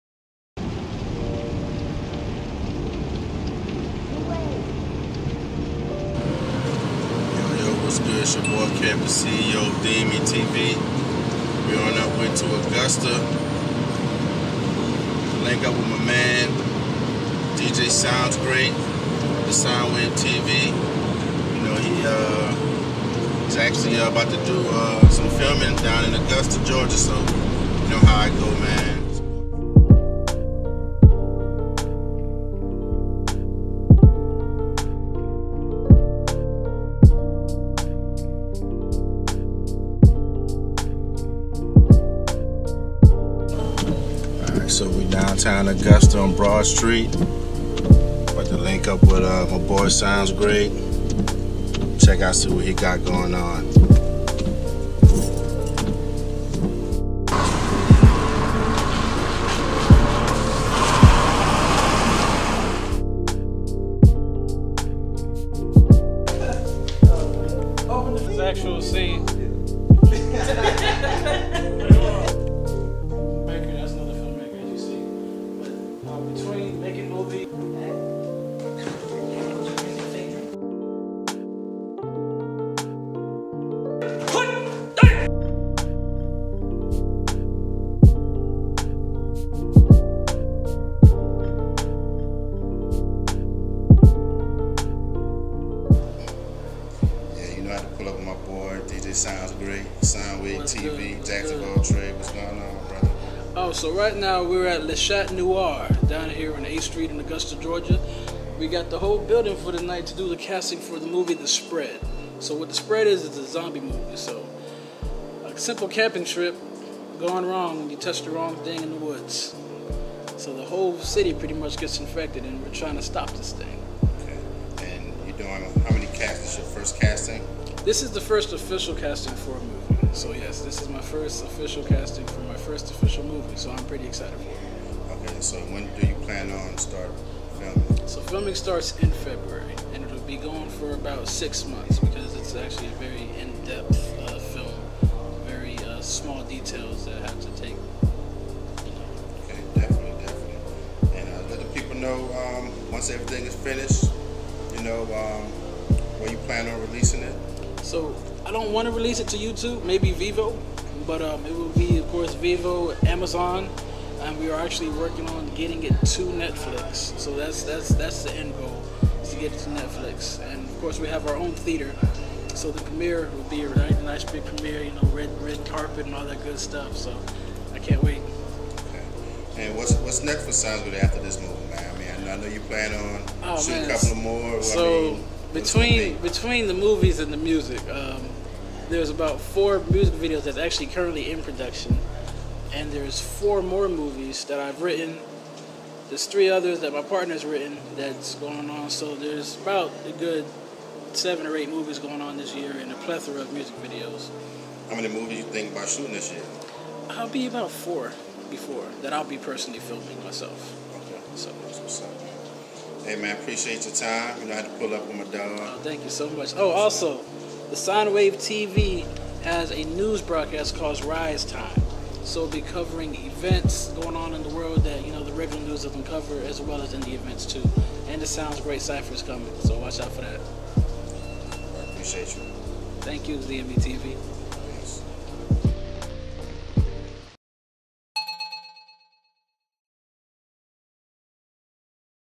VLOGCAP ROAD TO SOUNDZGRATE X DMEtv ( prod: NuWAV)
Check out KevDaCEo of DMEtv vlogcap of Corbin Media, LLC casting call for " The Spread" .

Casting call was held at Le Char Noir in Augusta, GA,